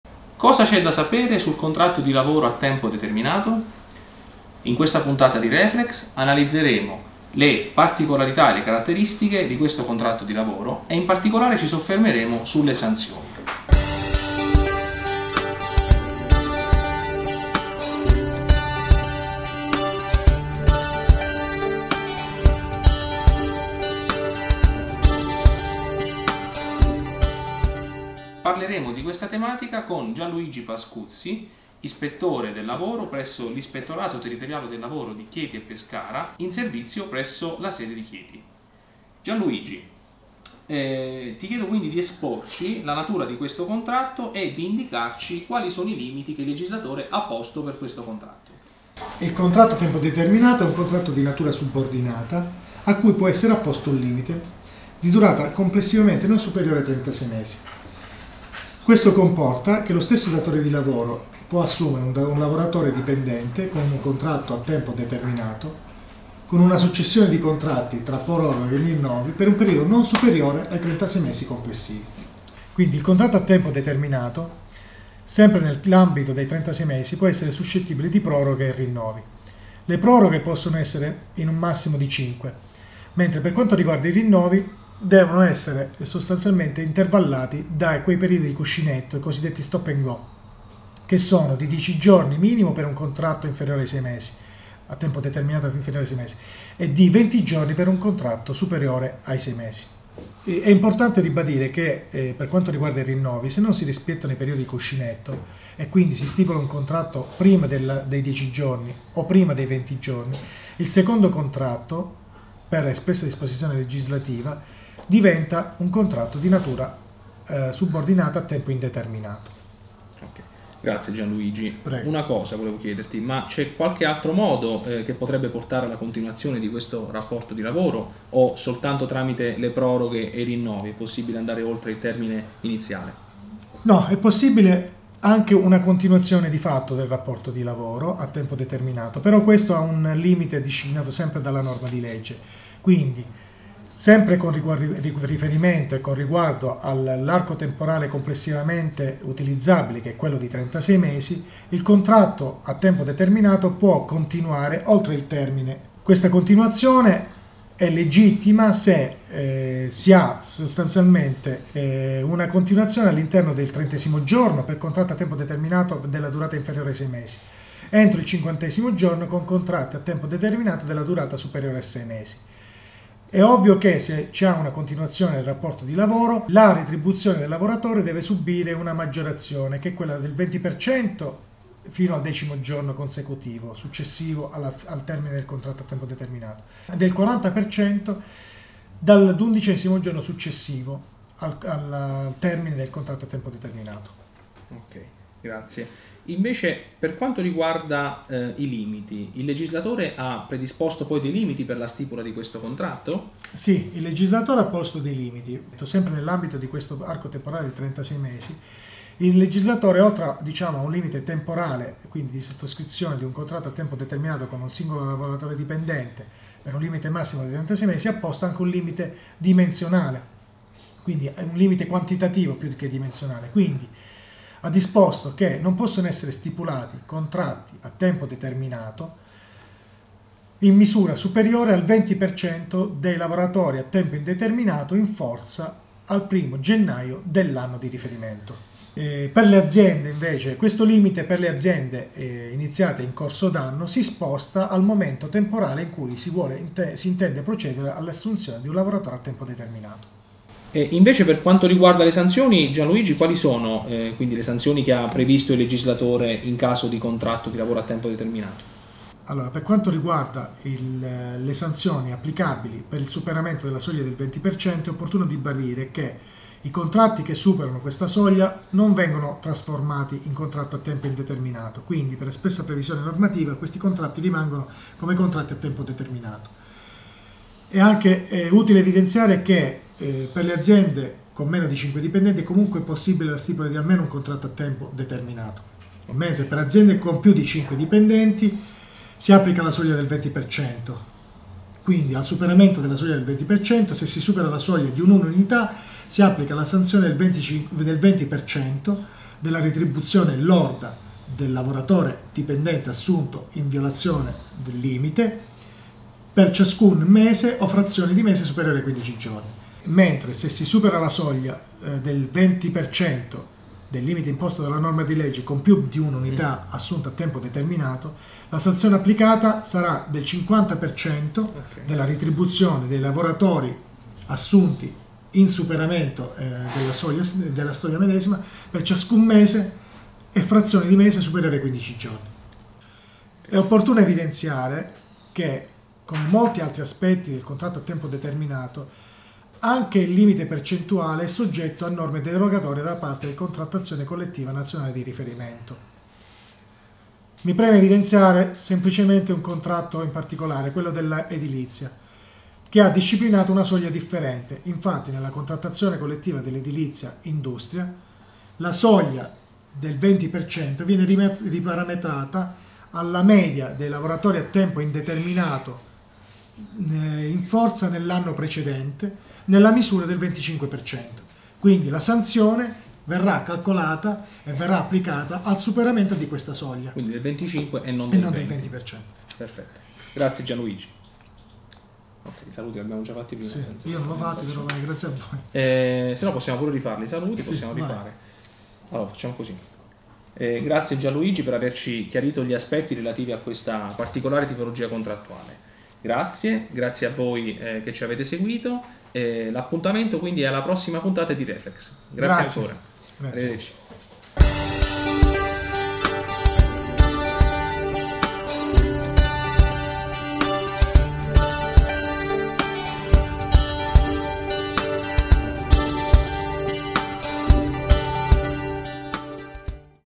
Contratto di lavoro a tempo determinato, cos'è e come funziona
Il contratto di lavoro a tempo determinato è una forma di contratto di lavoro che prevede un termine finale ovvero una durata prestabilita. Lo stesso può essere prorogato (anche con proroghe di fatto) e rinnovato ma non può superare la durata massima di 36 mesi. In questa video guida vediamo nel dettaglio come funziona, i limiti e le peculiarità.

▶ A cura di Informa360 e Lavoro e Diritti
▶ In video Dott. Gianluigi PASCUZZI (Ispettore del Lavoro ITL Chieti)
▶ In video Dott. Manuel MARINI (Consulente del Lavoro in Chieti)

Le considerazioni contenute nel presente video sono frutto esclusivo del pensiero dell’autore e non impegnano, in alcun modo, l’amministrazione di appartenenza.